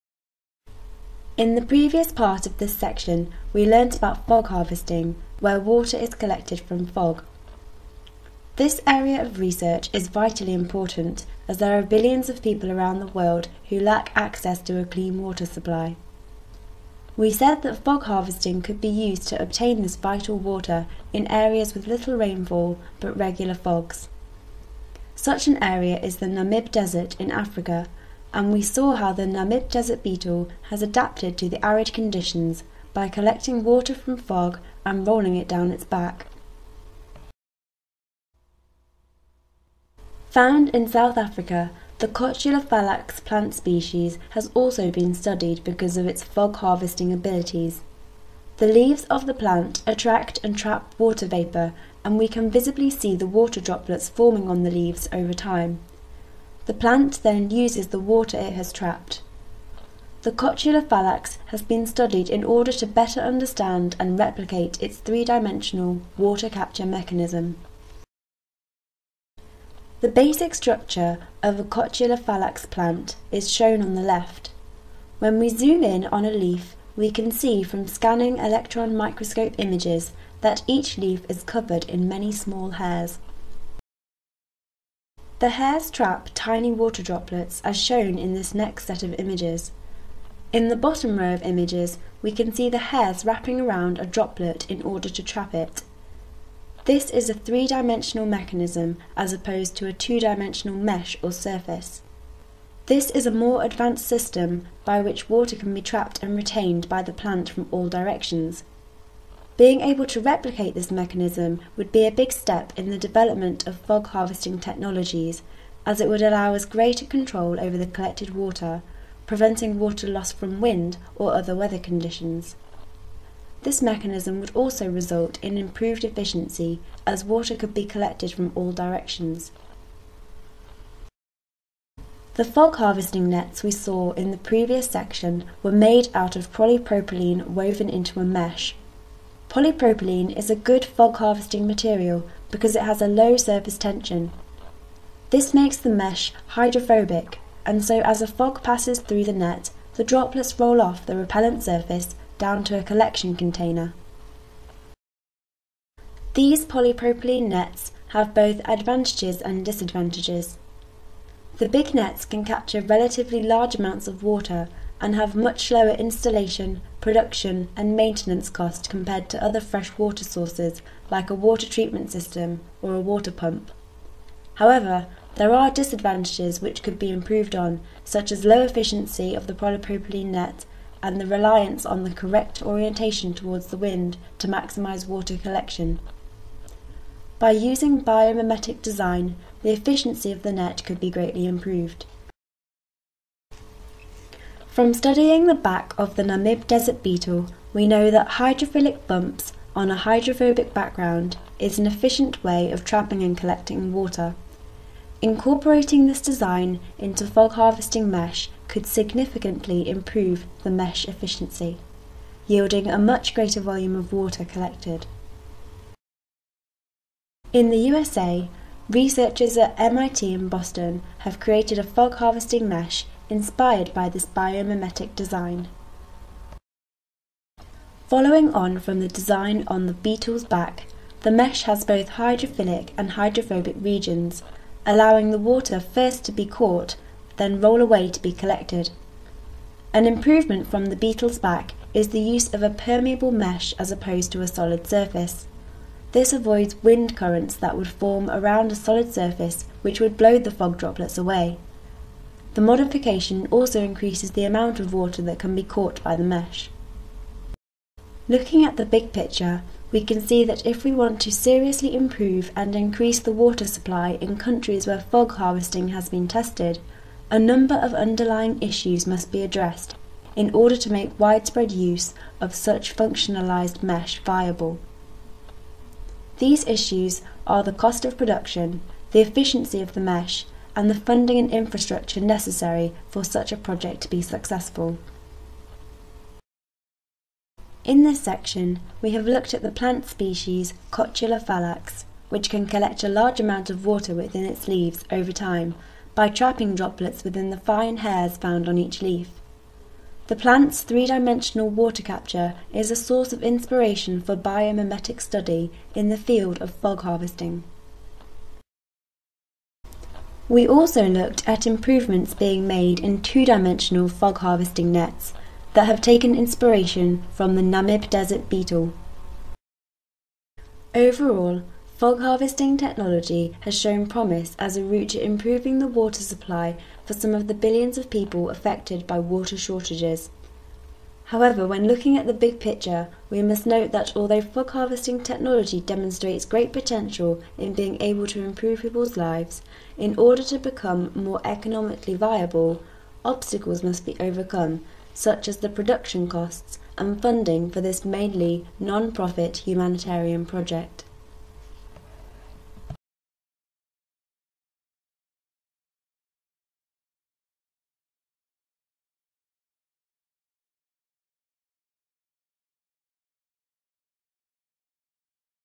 Functional Surfaces A4 - Fog Harvesting (Advanced)
In the last part of this section we looked at the Namib Desert Beetle. In this video we go further and study the Cotula Fallax plant species which has a 3-dimensional water capture mechanism. We also look at the issues faced by current fog harvesting nets and the next generation of biomimetic designs.
This is the second part of section 5 in this series of online video primers teaching about the behaviour of water on surfaces. This video is designed for ages 18+ (Undergraduate level).